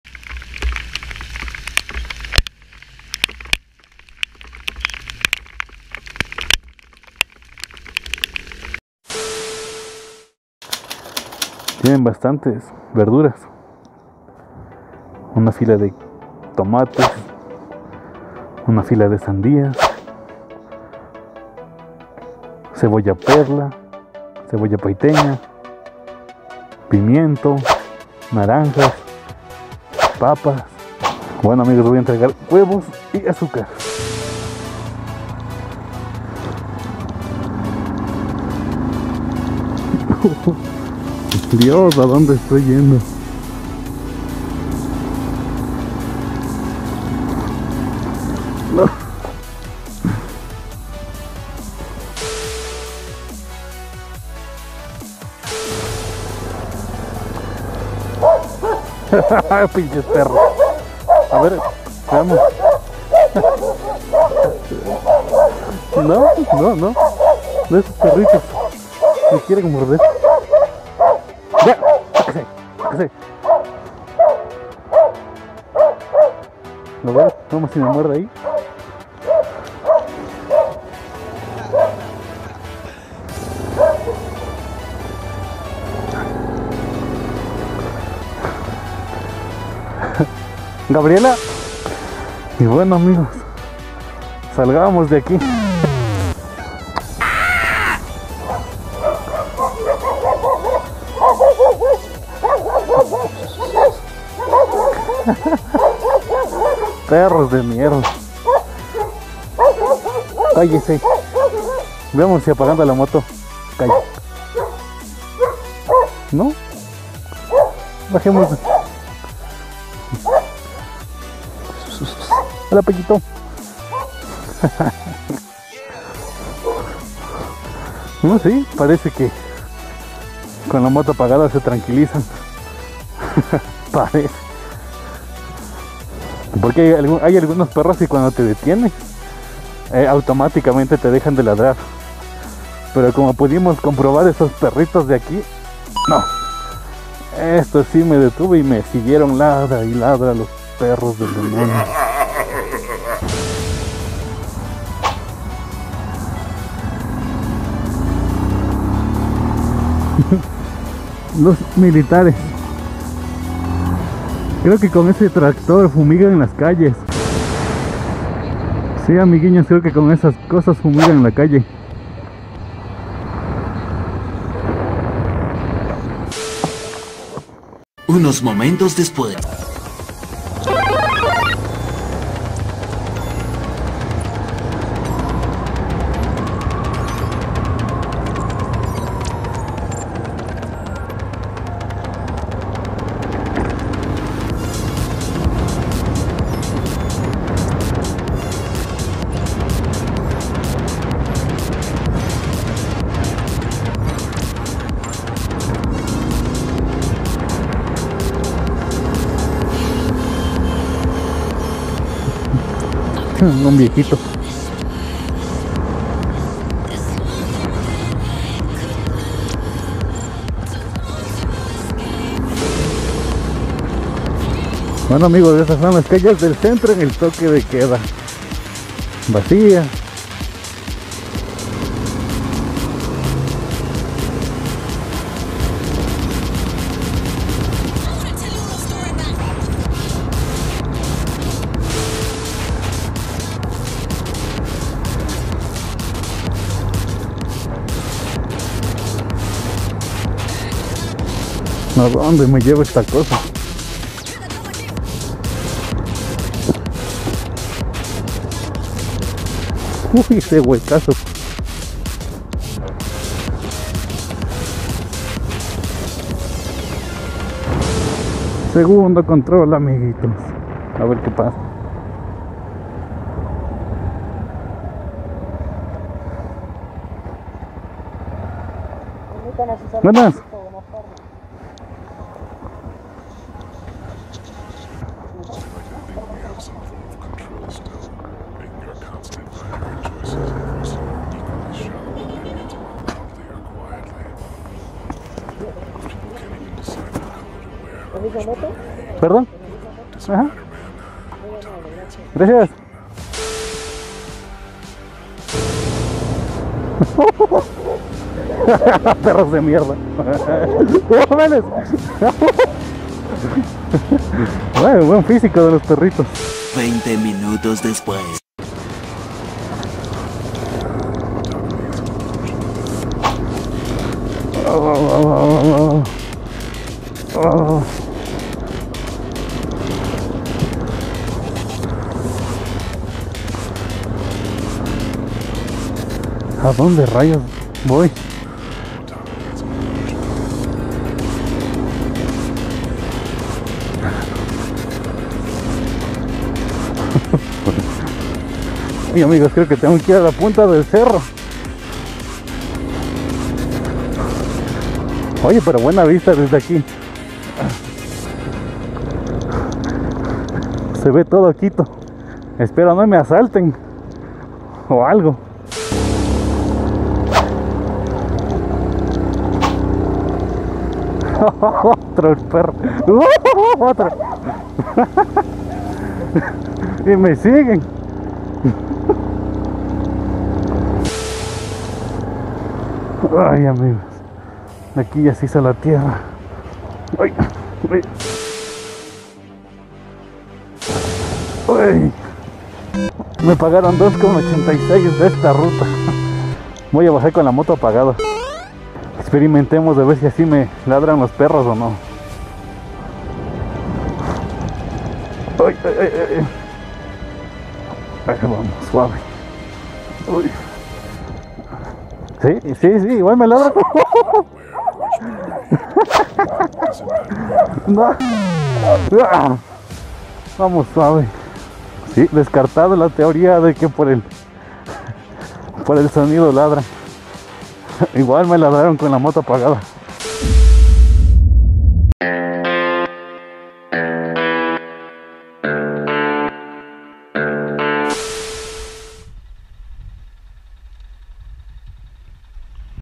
Toque de queda QUITO (PERROS, casi me comen )🐶 |Uber| 🍵 #5
Repartiendo comida con la aplicación de UBER.
Siempre llevo mi mascarilla y mi alcohol.
Cuídense.